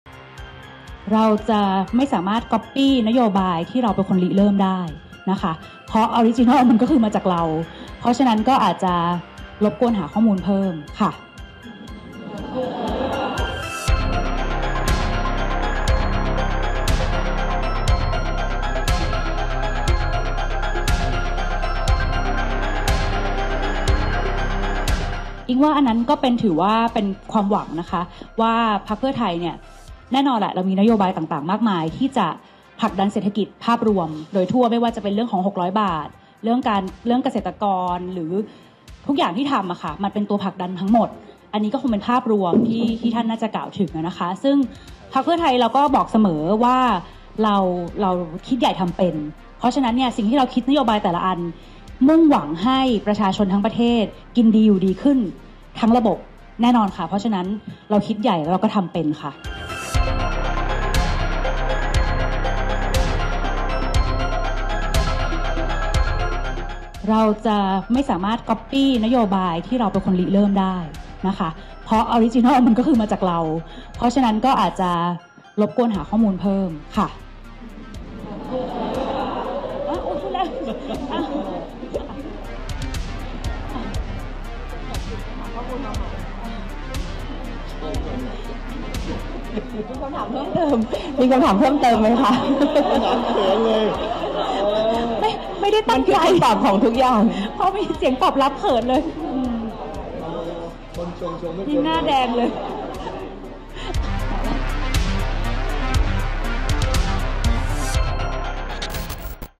อุ๊งอิ๊งค์ ฟาด “ภูมิใจไทย” ใครลอกใคร ? | NationNewsFeed
“อุ๊งอิ๊งค์” ฟาดกลับ “ภูมิใจไทย” จากกรณีกล่าวหาว่าลอกนโยบายพักชำระหนี้ ยัน ริเริ่มโดย “พรรคเพื่อไทย”

Line : @nationonline